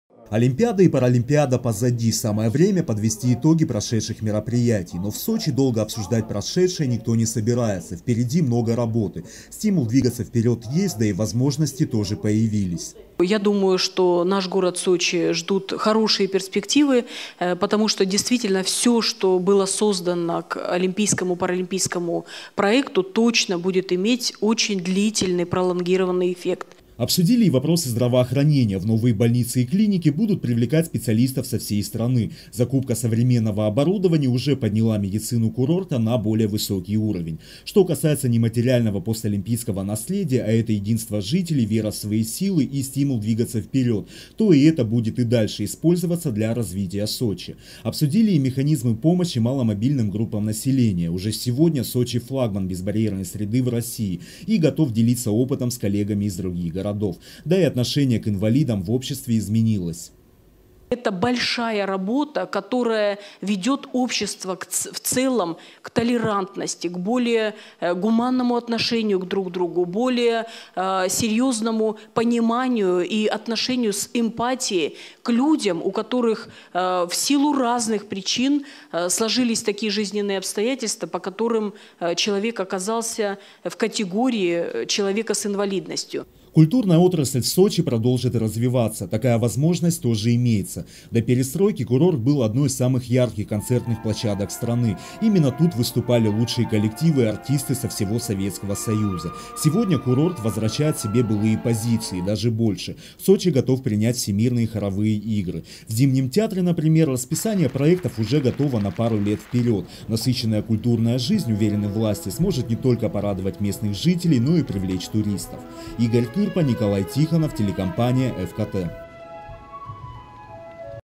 С журналистами встретилась заместитель главы Сочи Ирина Романец. Новости 24 Сочи
Журналистов интересовала судьба безбарьерной среды, кто и как будет обслуживать эту инфраструктуру. Разговоры коснулись и темы развития здравоохранения и культурной жизни Сочи.
Сочи. Новости.
Телеканал Эфкате.